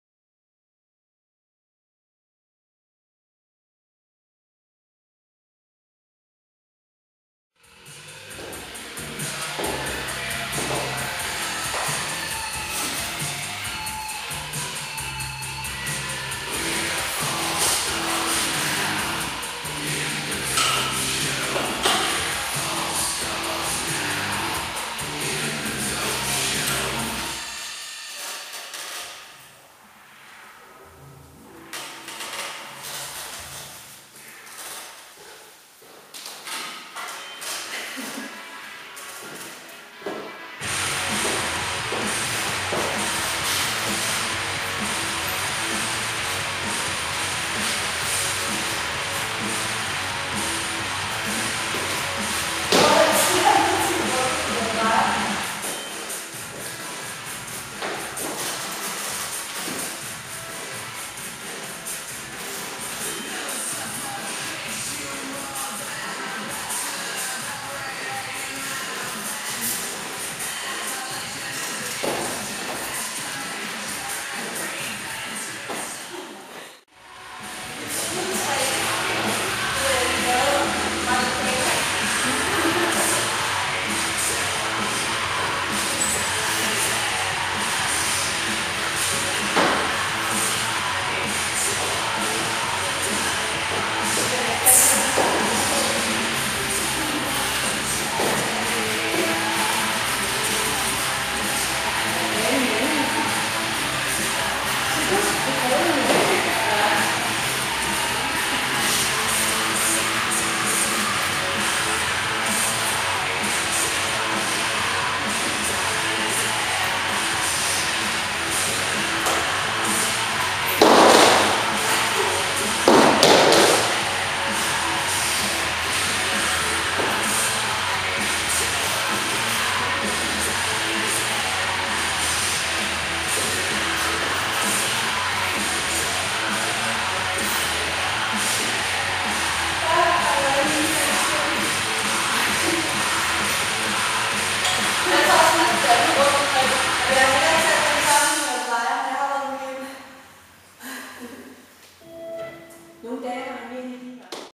Charlotte freestyler - Fanny træner combo (31-08-2011)
Charlotte Peippo freestyler imens Fanny træner på sit sammensatte show til dm i poledance 2012

Peippos pole studio i Nykøbing Sjælland, instruktør Charlotte Peippo